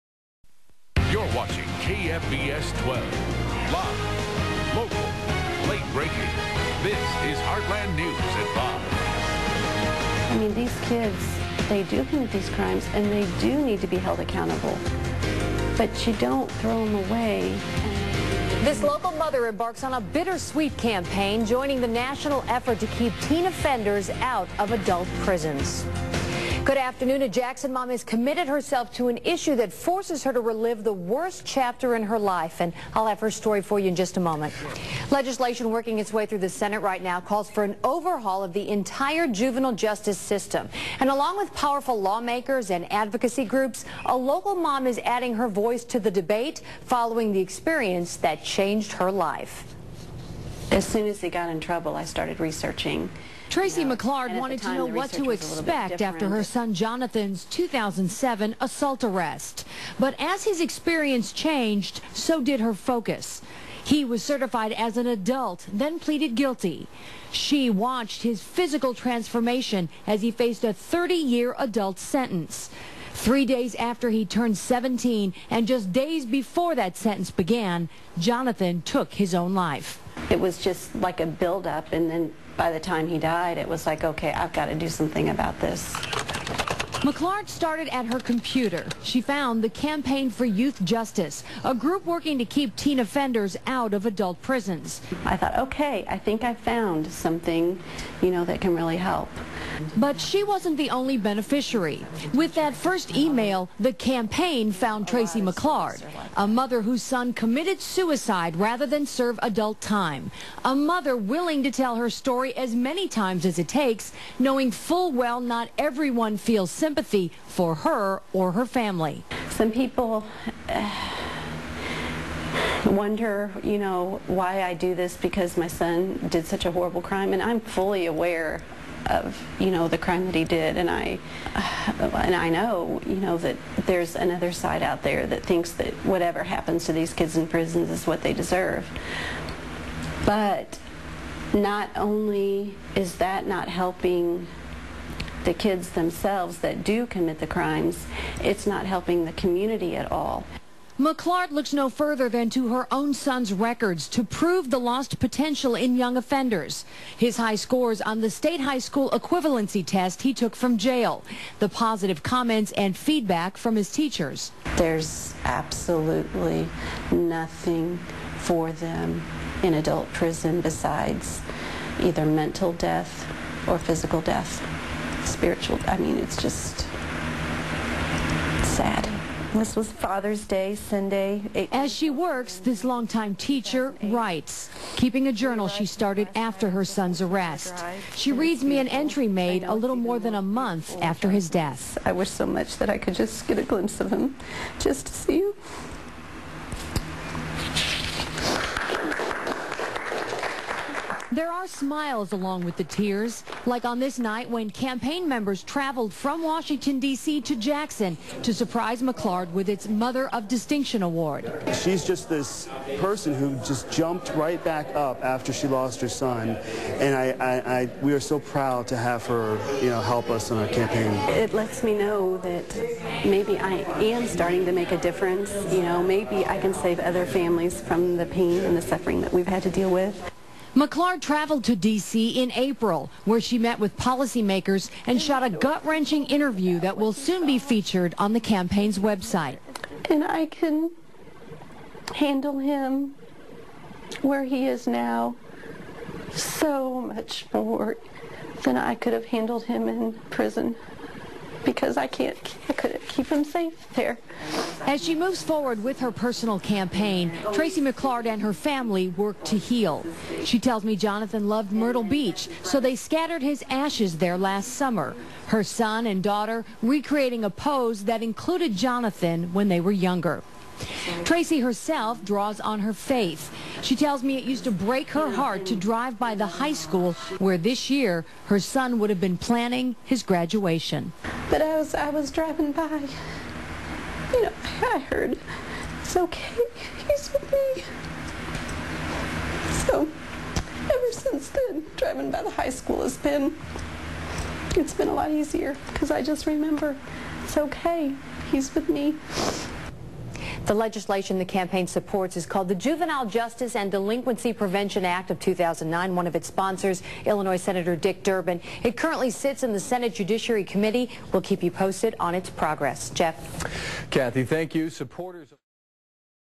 KFVS TV Feature Story on Tracy McClard
Tracy McClard, a Jackson, MO mother, lost her son to suicide in an adult jail. KFVS-TV profiles this incredible mother and her juvenile justice advocacy work.